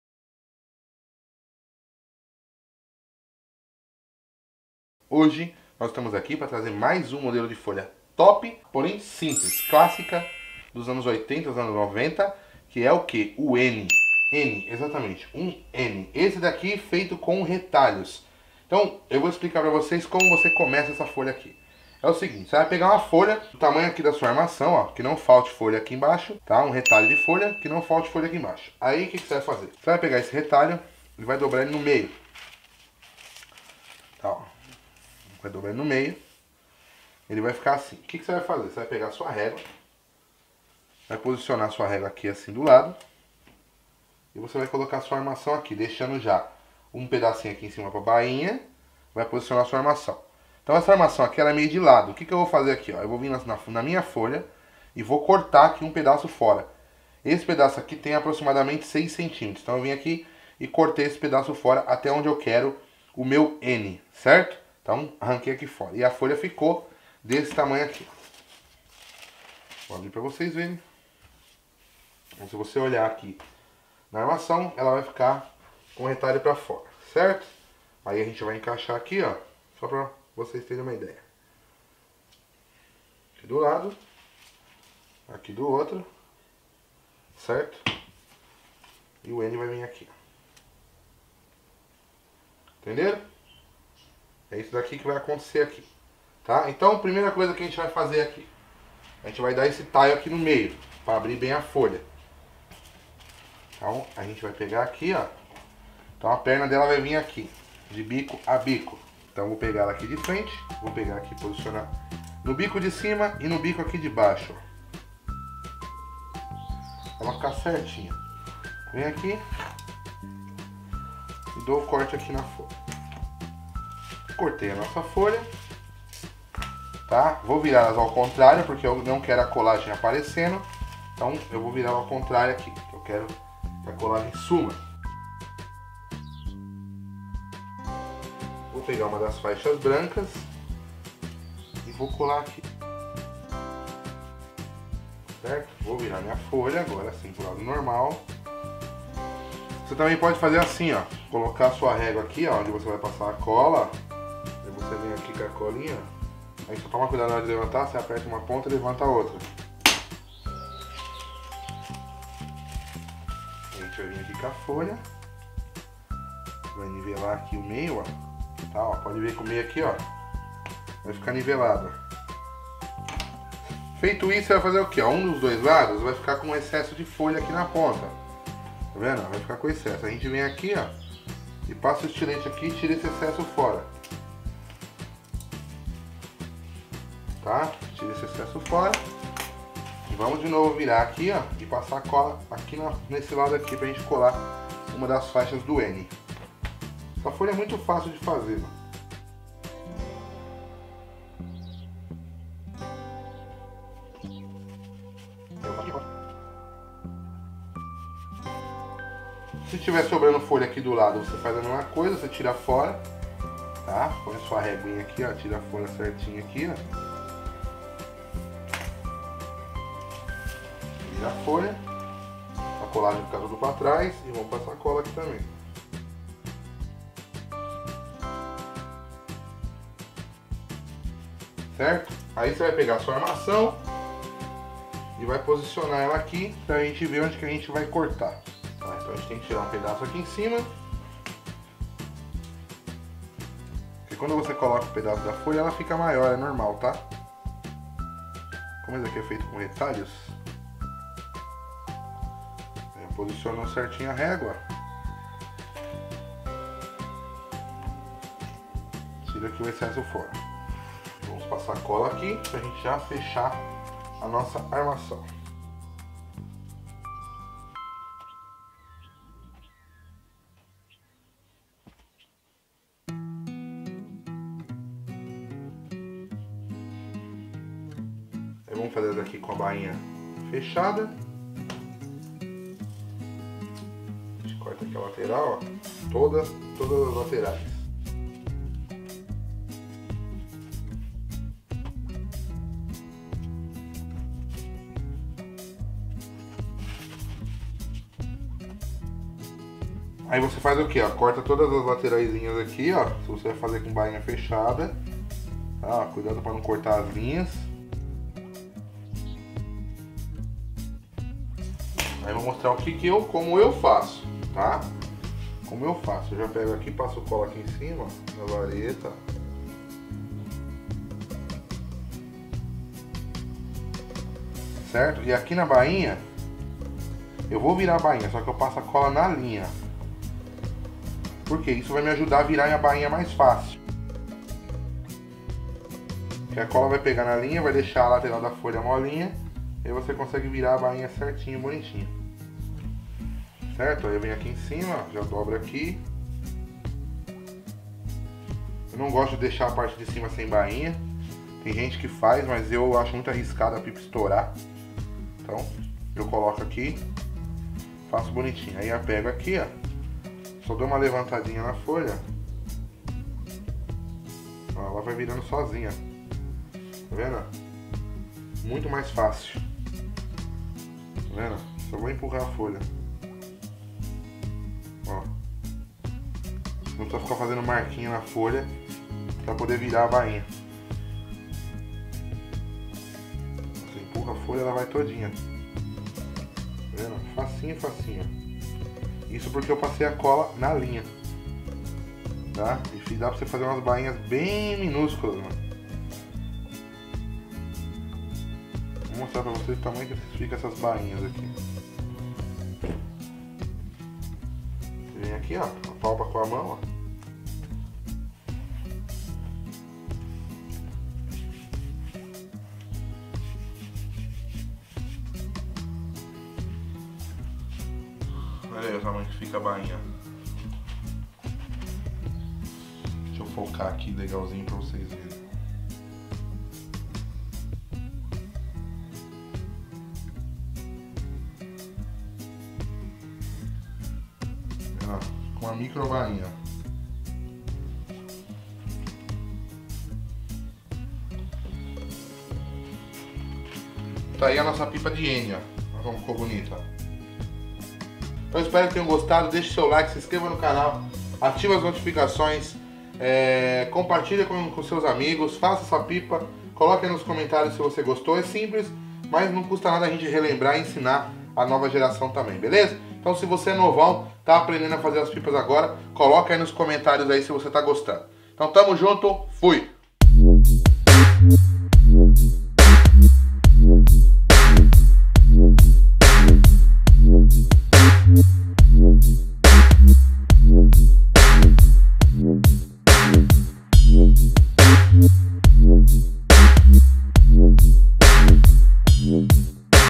Pipa Corte Recorte em N Simples / Escola de Pipeiros
Hoje vamos ensinar mais um papel de pipa corte recorte simples das antigas. Esse é bem clássico e faziamos com retalhos de folha de pipa.
Segue como fazer corte recorte em N.